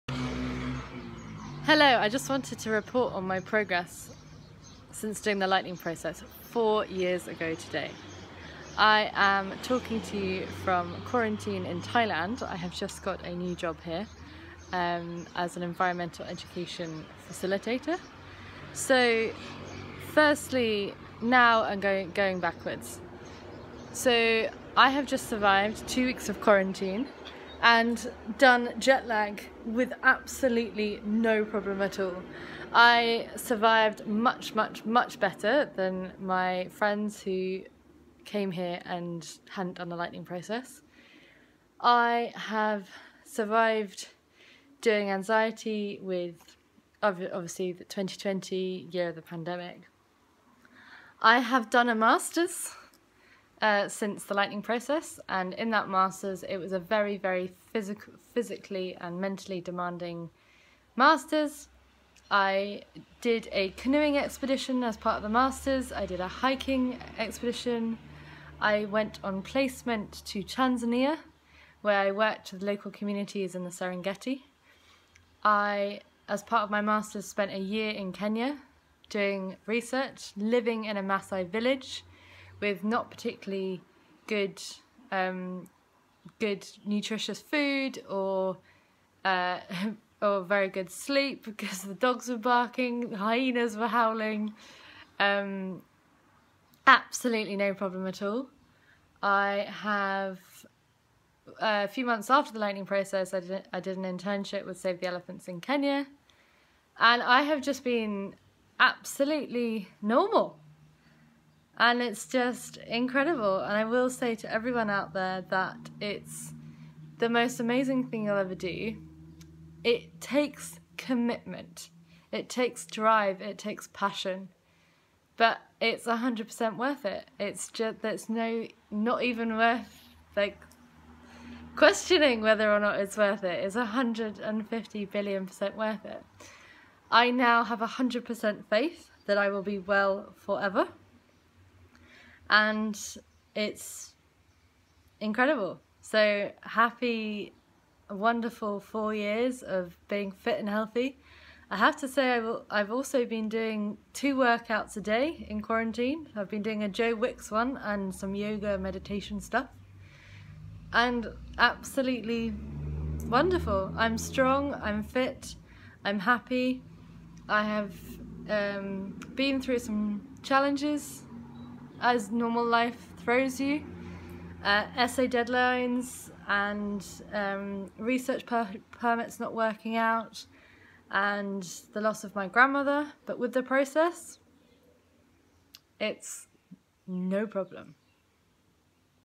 Does recovery from ME/CFS last?
Georgie recovered from ME/CFS 4 years ago using the Lightning Process with Helen Harding, who is part of Phil Parker's London team. She made a video during the LP, this is her 4 year update on life after ME/CFS